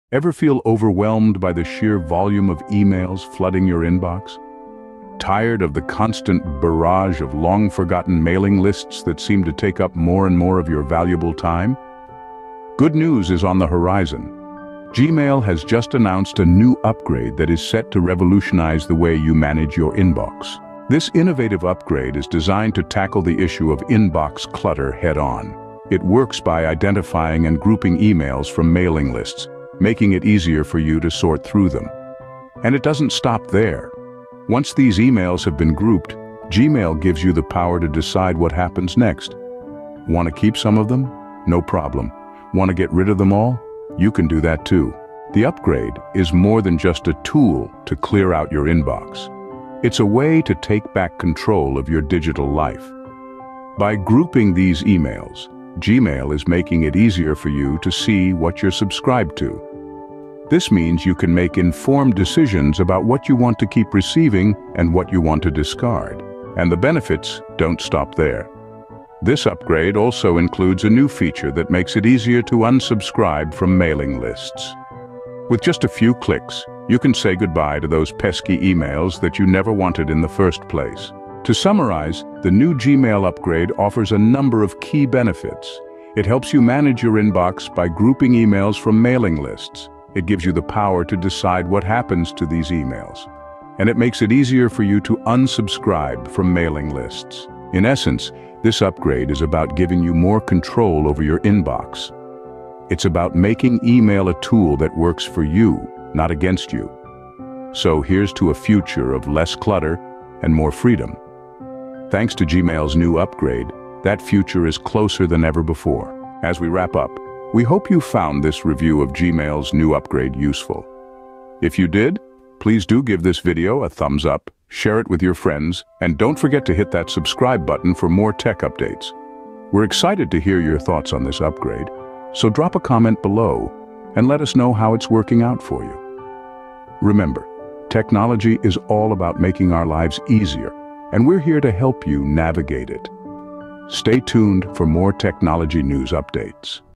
Gmail's Inbox Revolution: Cleaner and Better!
Introducing Gmail's latest game-changing upgrade: a cleaner inbox experience like never before! Say goodbye to clutter and hello to seamless organization with GmailCleanUp. This upgrade revolutionizes your email management, offering a clutter-free interface that enhances productivity and focus. Navigate effortlessly through your inbox with StreamlinedExperience, making every interaction smoother and more efficient. With SmartSorting, important emails are prioritized, ensuring you never miss a crucial message again. Experience the power of OrganizedInbox, where emails are neatly categorized, allowing you to find what you need in a snap. Embrace the future of email with Gmail's Cleaner Inbox upgrade – because a clutter-free life is just a click away.

OUTLINE:
00:00:00
Introduction to Gmail's New Upgrade
00:00:20
Grouping and Managing Emails
00:00:45
Taking Back Control of Your Digital Life
00:01:06
Easy Unsubscribe Feature
00:01:21
Summary of Key Benefits
00:01:37
Empowering Users with Control
00:01:56